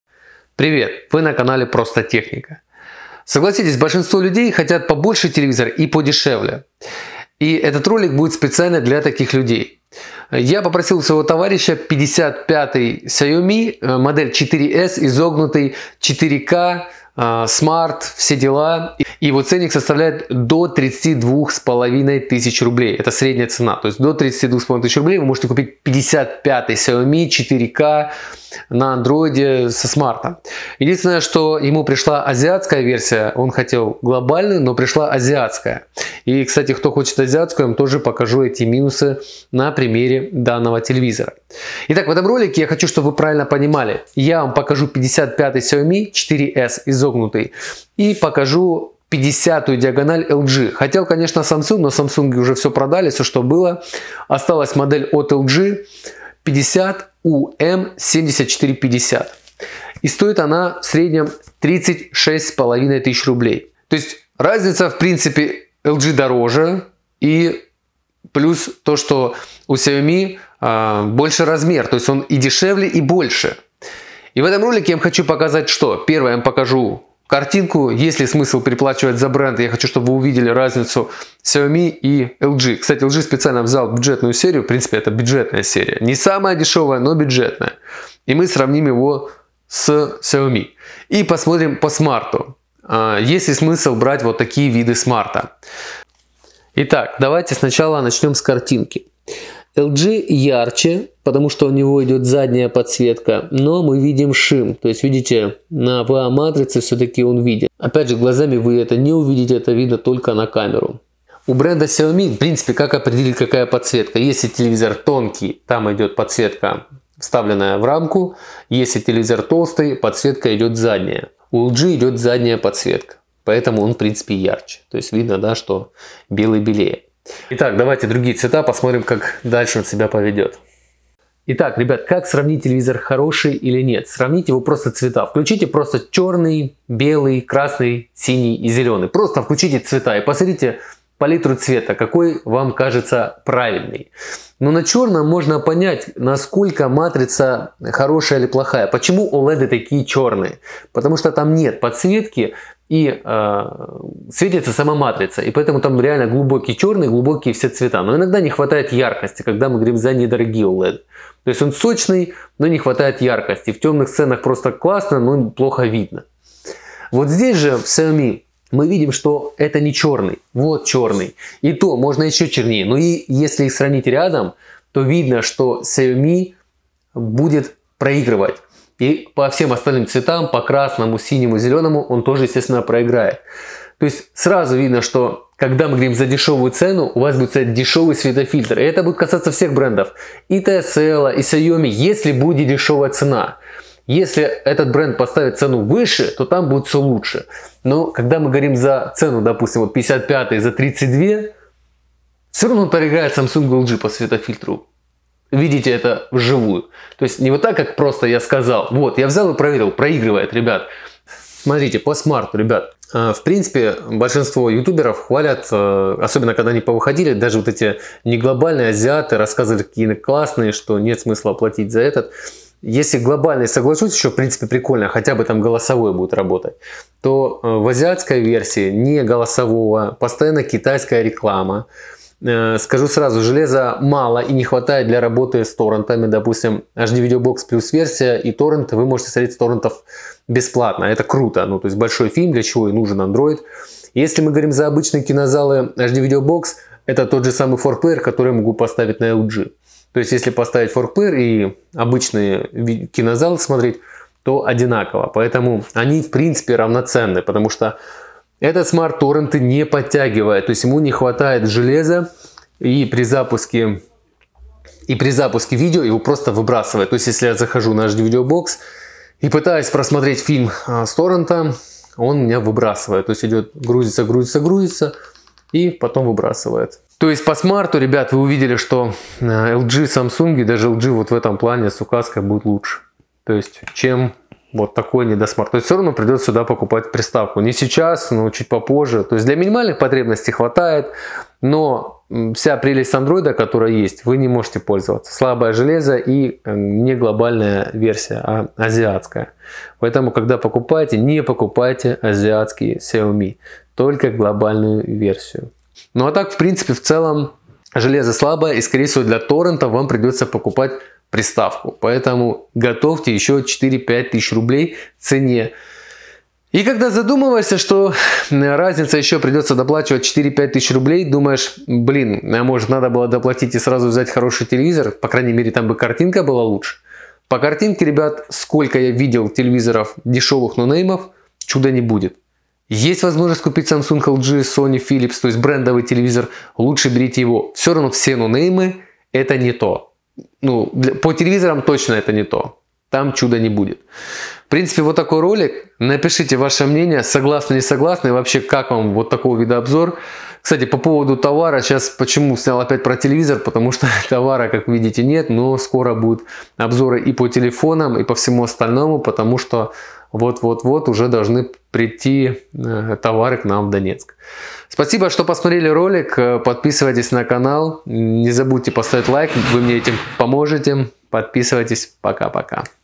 55 диагональ Xiaomi 4S Curved(азиат) против LG 50UM7450! Лучше 50 диагональ,но бренд или 55 Xiaomi
Небольшой тест телевизоров Xiaomi 55 4K против 50 LG 4K
Xiaomi стоит 32000 р
LG стоит 36500 р

Я считаю лучше взять меньше, но лучше по картинке, чем больше, но с корявым смартом и менее сочной картинкой! А вы?